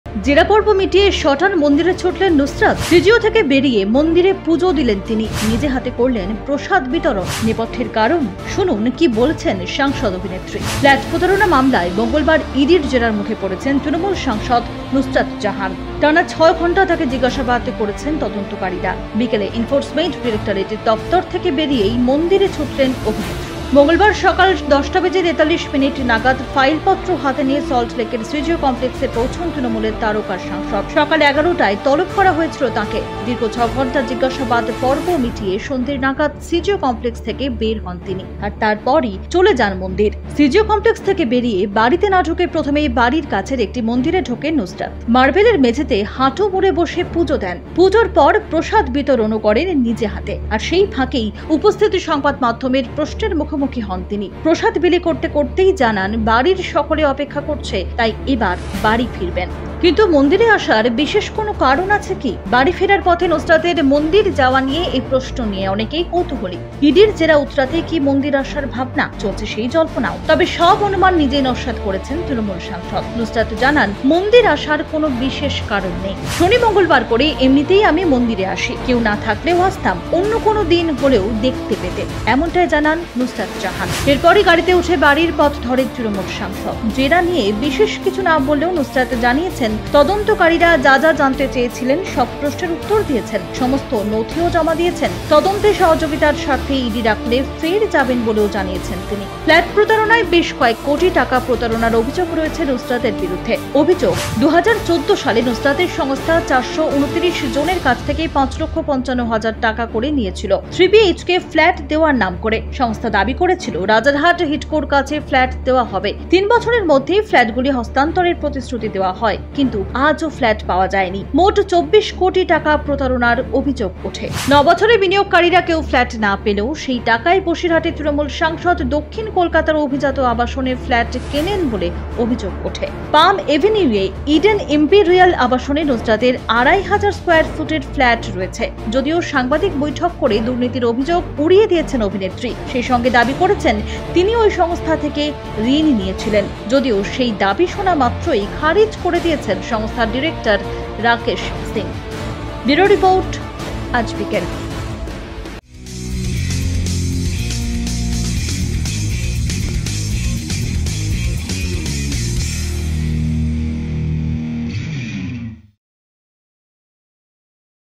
ED-র জেরা শেষ হতেই মন্দিরে ছুটলেন নুসরত! হাঁটু মুড়ে মাটিতে বসে পুজো
জেরা পর্ব মিটিয়ে সটান মন্দিরে ছুটলেন নুসরত!
‘সিজিও’ থেকে বেরিয়ে মন্দিরে পুজো দিলেন তিনি
নিজে হাতে করলেন প্রসাদ বিতরণ…
নেপথ্যের কারণ? শুনুন কী বলেছেন সাংসদ অভিনেত্রী
 সিজিও কমপ্লেক্স থেকে সটান মন্দিরে!
 মন্দিরে পুজো দিলেন নুসরত জাহান
 নিজেই জানালেন নেপথ্যের ‘কারণ’
 জেরা শেষে ‘ভক্ত’ নুসরত